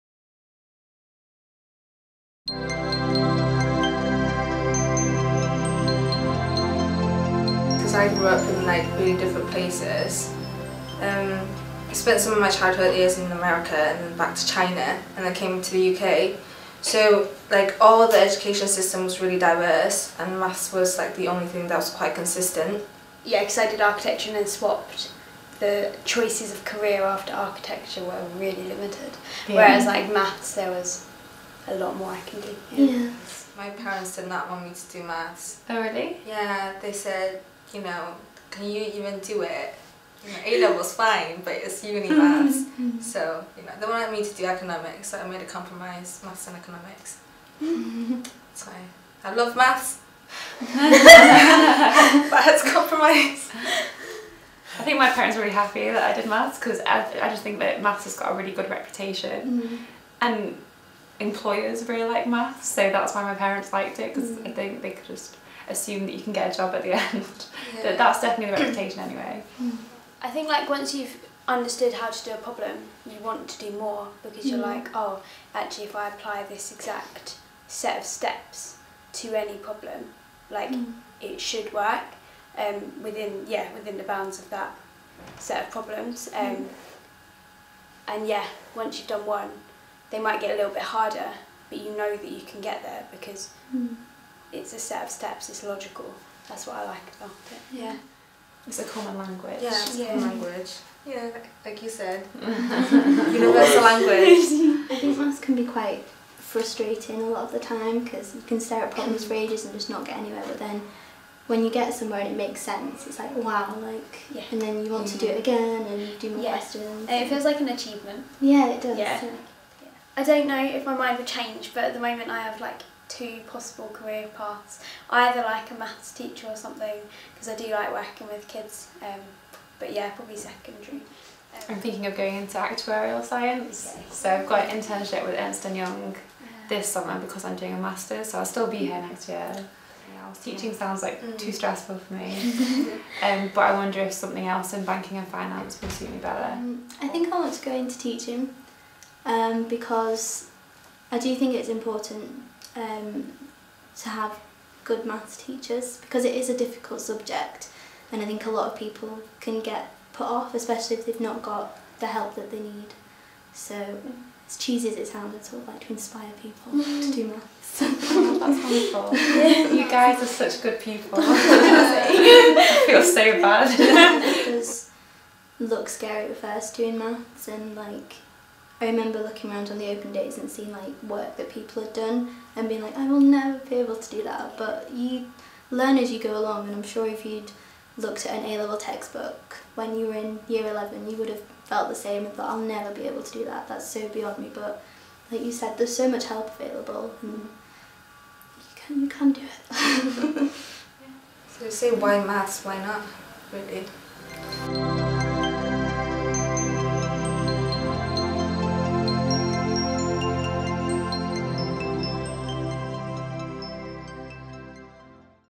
Women in Maths - The Undergraduate Students
Some undergraduate students from the School of Mathematical Sciences chat about why they chose to study maths.